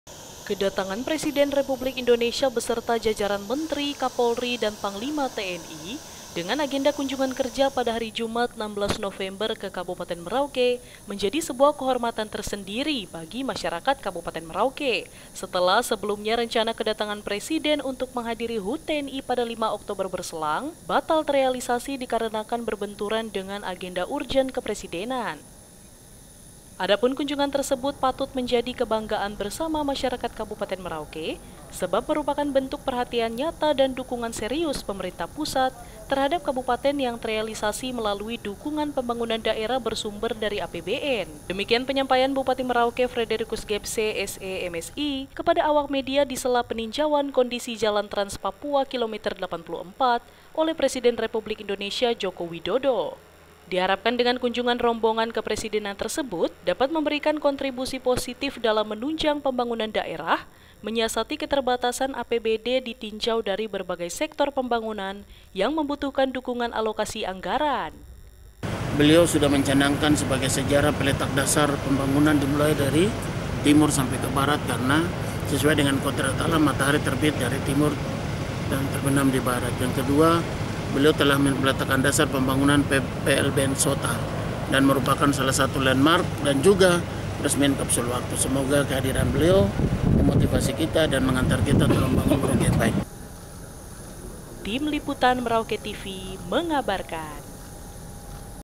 BUPATI MERAUKE APRESIASI KUNJUNGAN PRESIDEN JOKOWI
KEDATANGAN PRESIDEN REPUBLIK INDONESIA BESERTA JAJARAN MENTERI KAPOLRI DAN PANGLIMA TNI DENGAN AGENDA KUNJUNGAN KERJA PADA HARI JUMAT 16 NOVEMBER KE KABUPATEN MERAUKE MENJADI SEBUAH KEHORMATAN TERSENDIRI BAGI MASYARAKAT KABUPATEN MERAUKE SETELAH SEBELUMNYA RENCANA KEDATANGAN PRESIDEN UNTUK MENGHADIRI HUT TNI PADA 5 OKTOBER BERSELANG BATAL TEREALISASI DIKARENAKAN BERBENTURAN DENGAN AGENDA URGENT KEPRESIDENAN.